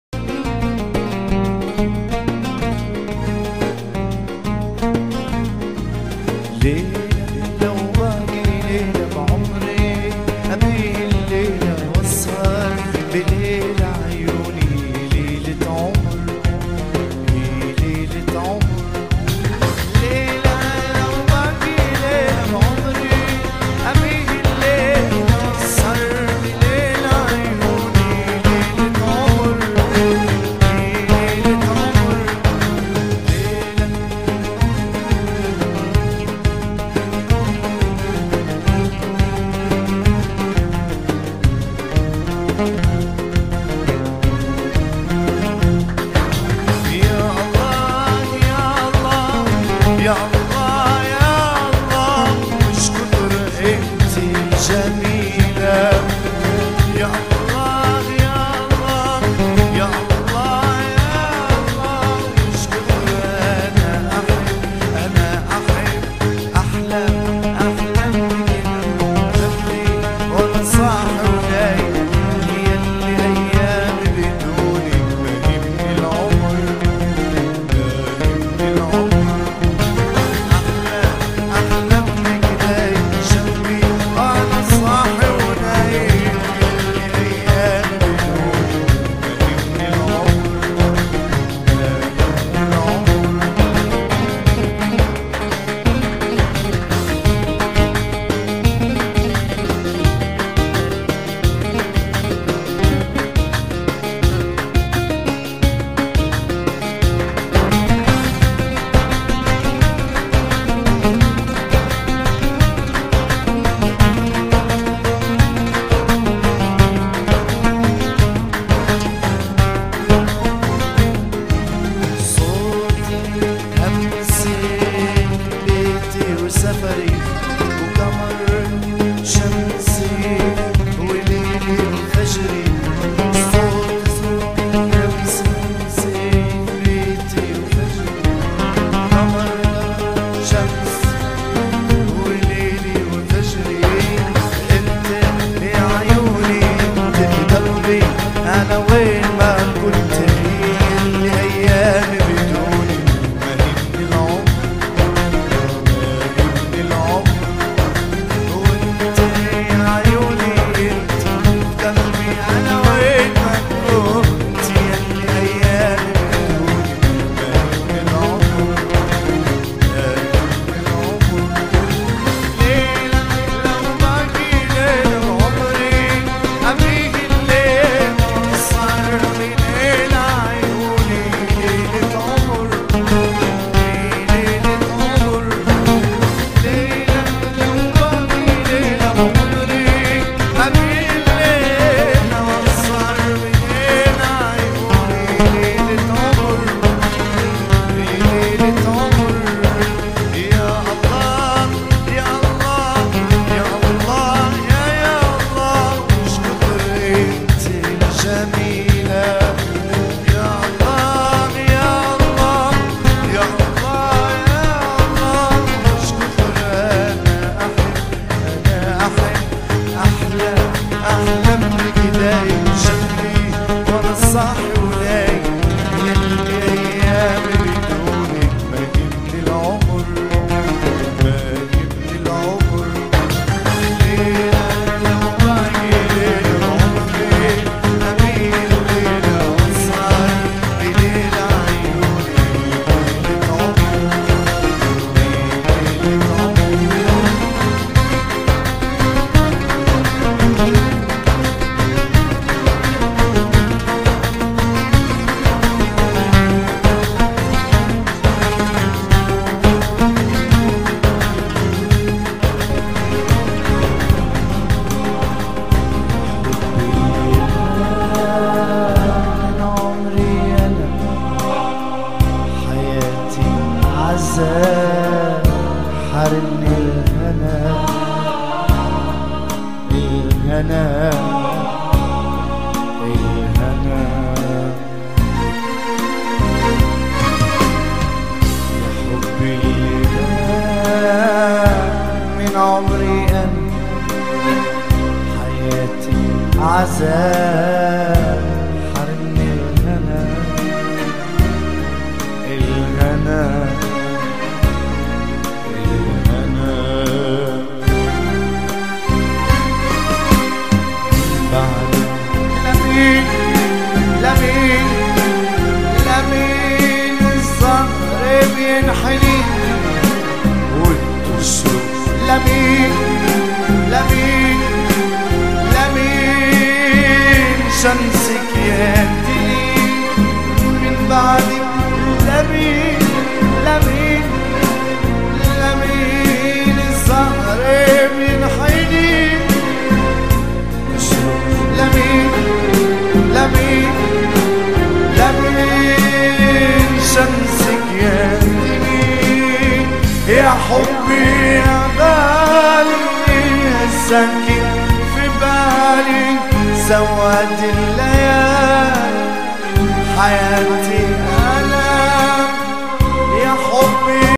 فرقة دريمز بيت لحم
فرقة دريمز الموسيقية بيت لحم ... بقيادة عازف الاورغ ابو وديع